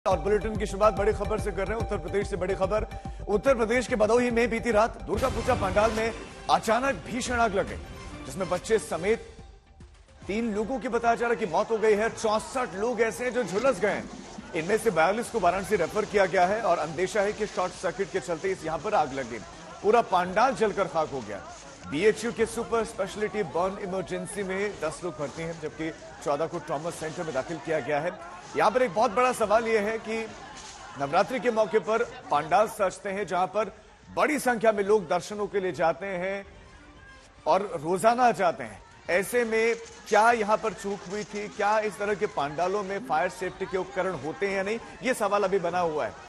UP: यूपी के भदोही में पूजा पंडाल में भीषण आग, आग में एक बच्चे समेत तीन लोगों की मौत | Latest News
यूपी के भदोही में बीती रात दुर्गा पूजा पंडाल में भीषण आग लग गई. बच्चे समेत तीन लोगों की मौत हो गई जबकि 64 लोग झुलस गए. इनमें से 42 को वाराणसी रेफर किया गया है. अंदेशा है कि शॉर्ट सर्किट से आग लगी। पूरा पंडाल खाक हो गया. बीएचयू के सुपर स्पेशलिटी बर्न इमरजेंसी में 10 लोग भर्ती है जबकि 14 को ट्रामा सेंटर में दाखिल किया गया है.

A fire broke out in Durga Puja pandal in Bhadohi, UP last night. Three people, including a child, died while 64 people were scorched. 10 people are admitted to BHU's Super Specialty Burn Emergency while 14 have been admitted to the Trauma Center.